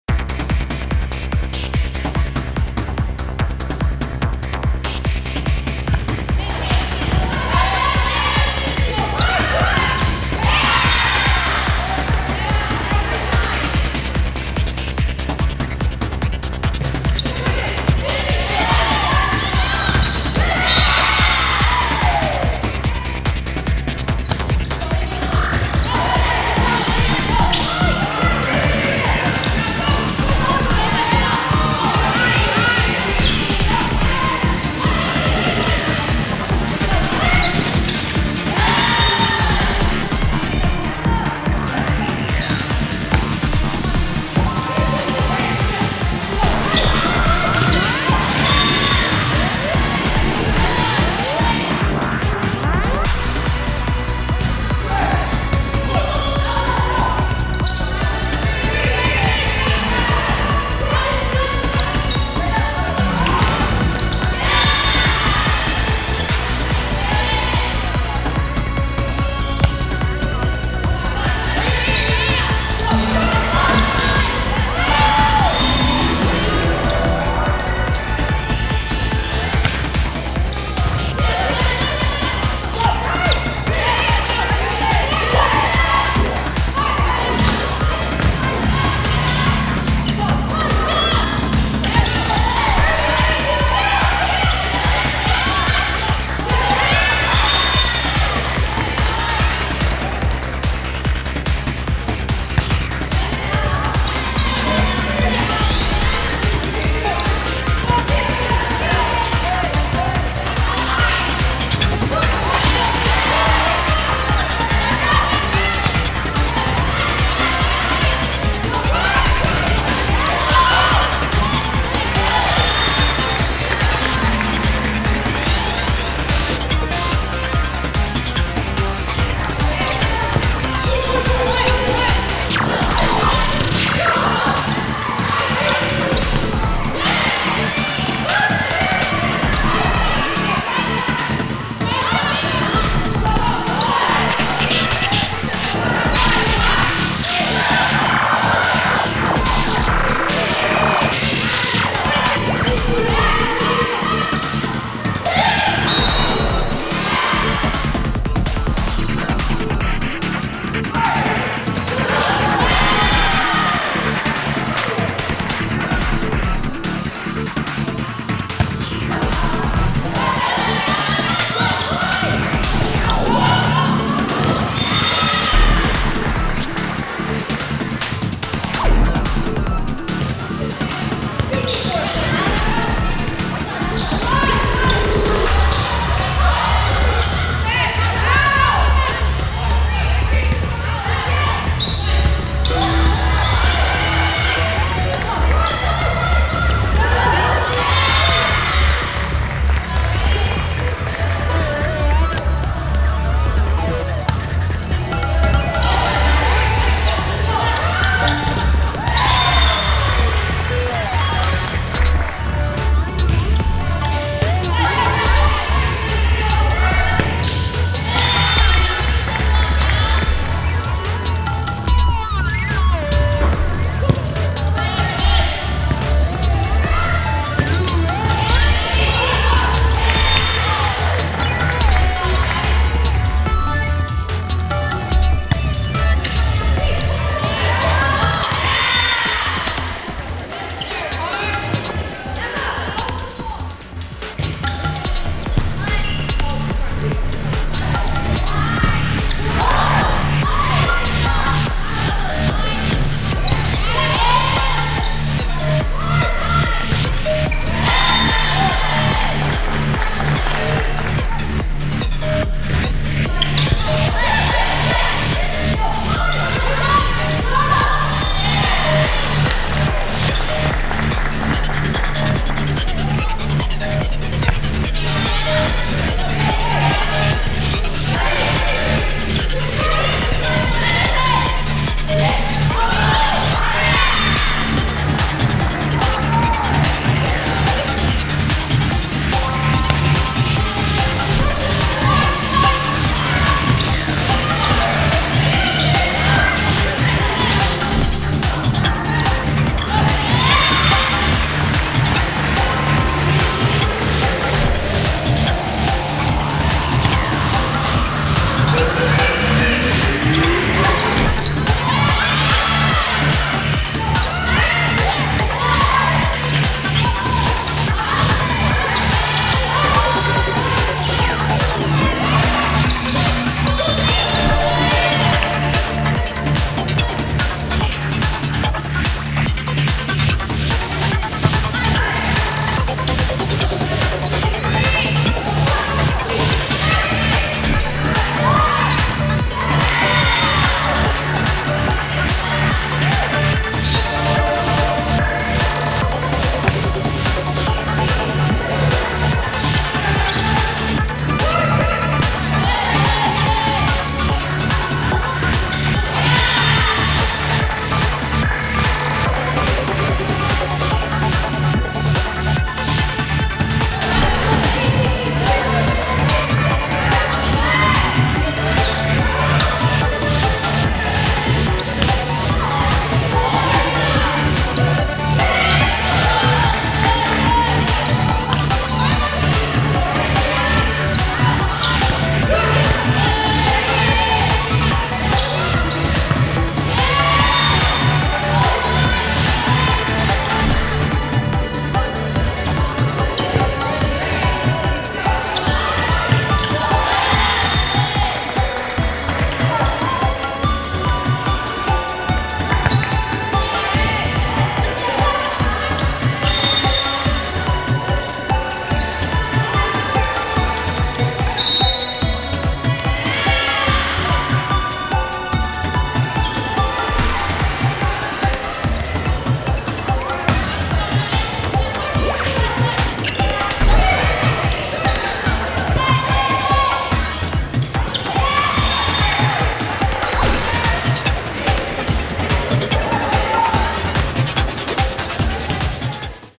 High School Volleyball: Kickapoo vs Incarnate Word
Bentonville High School ( Early Bird Tournament ) Volleyball Highlights:  Kickapoo vs Incarnate Word